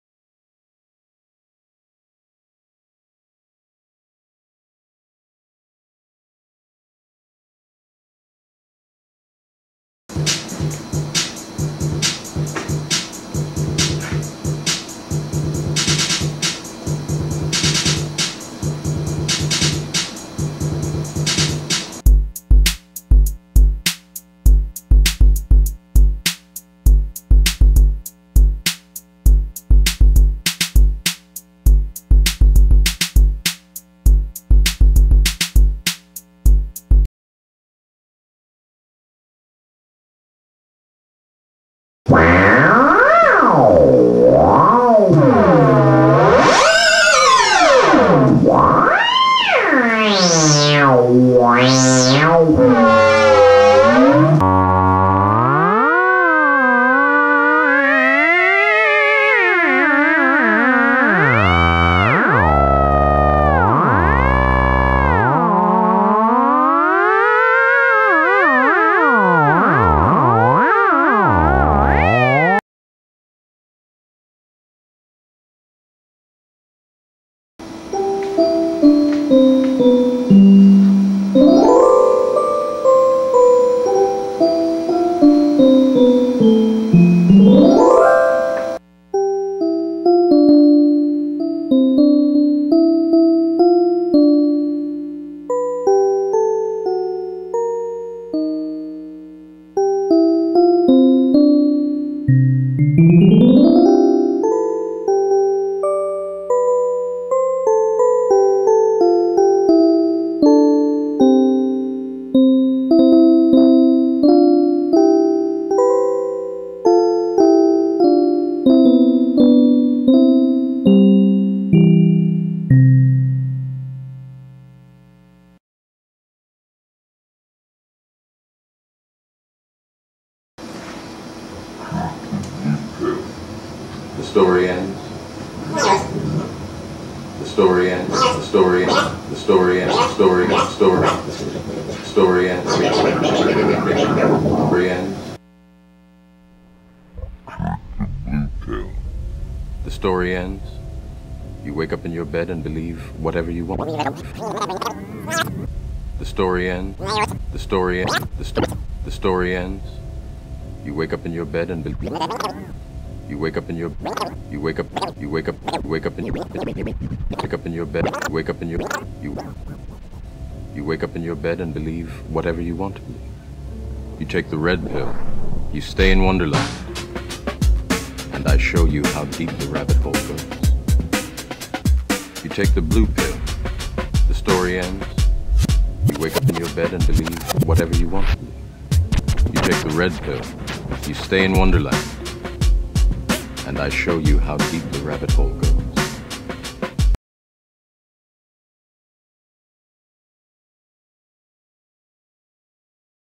Virtual Vibraphone (2004)
This is the video demonstration of the work I did at the University of Illinois at Urbana-Champaign. The demo consists of several virtual musical instruments. Originally recorded in 2004.